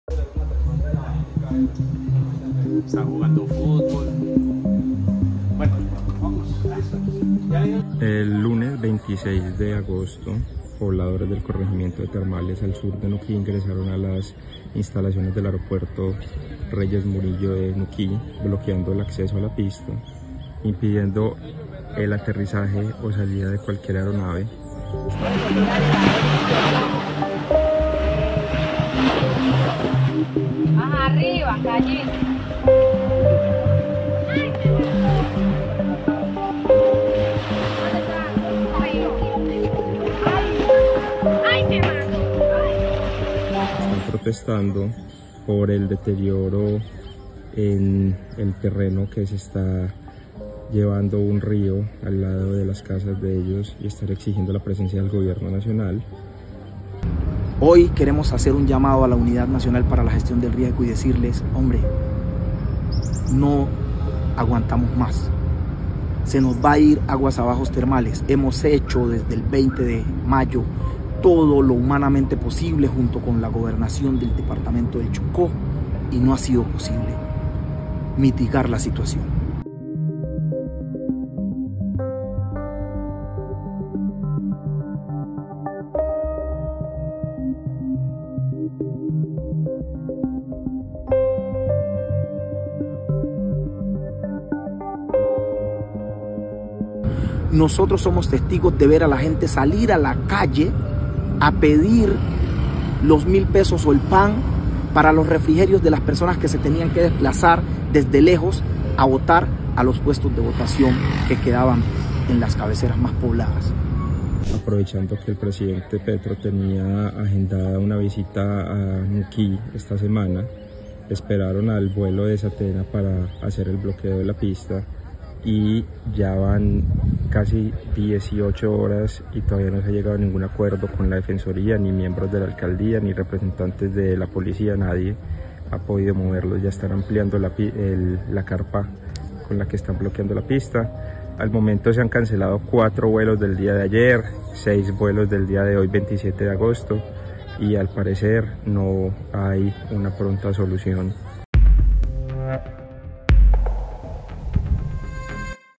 ¿Por qué está bloqueado el aeropuerto de Nuquí? | El Colombiano
Desde hace tres meses el río se ha llevado tres casas y ya amenaza con arrastrar la iglesia, un jardín infantil y 8 viviendas más. Este lunes, habitantes se manifestaron en la pista del aeropuerto en busca de ayuda.